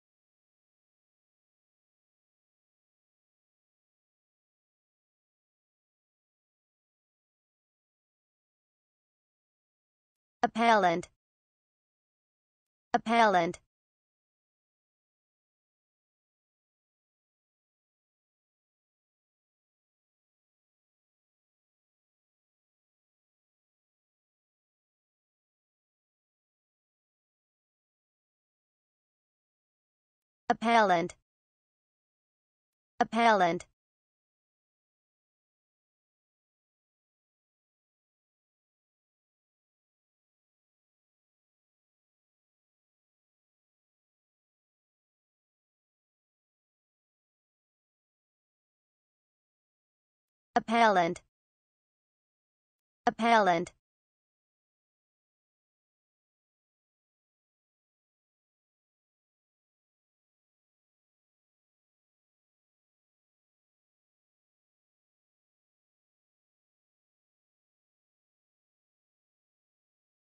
How to say Appellant in English? - Pronunciation Owl
How to say Appellant in English? Learn the pronunciation of Appellant!